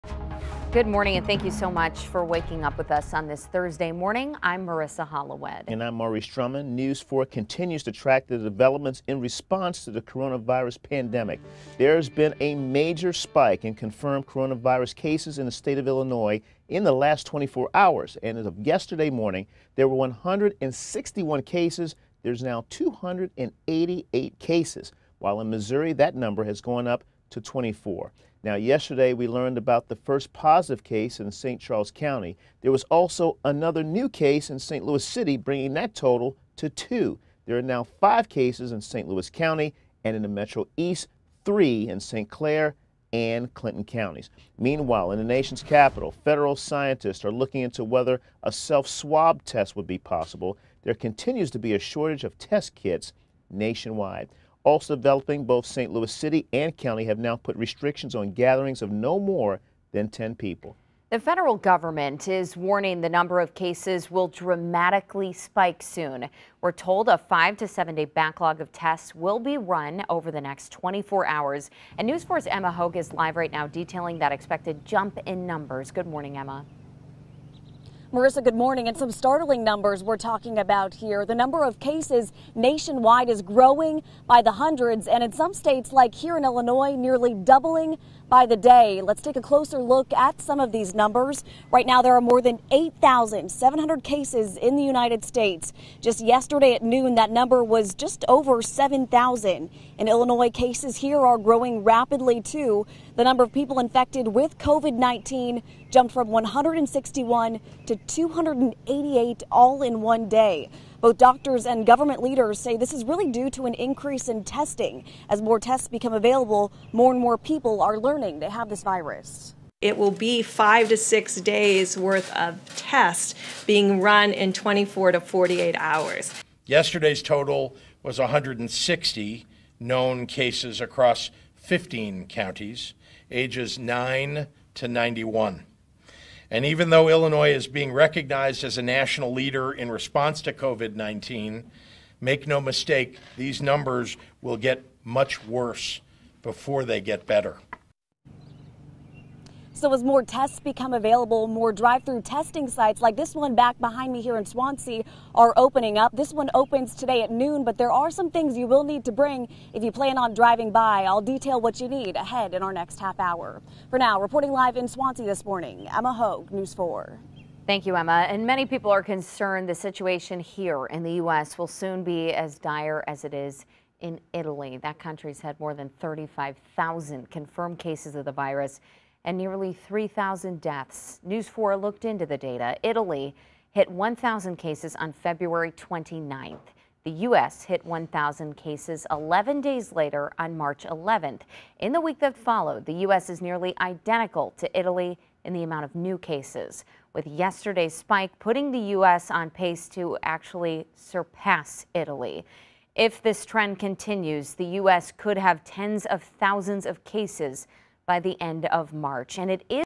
COVID-19 cases expected to rise as more tests become available
The federal government has warned that the number of cases will dramatically spike soon. In a press conference Wednesday, the coronavirus task force said there is a backlog of tests from five to seven days that will run in the next 24 hours.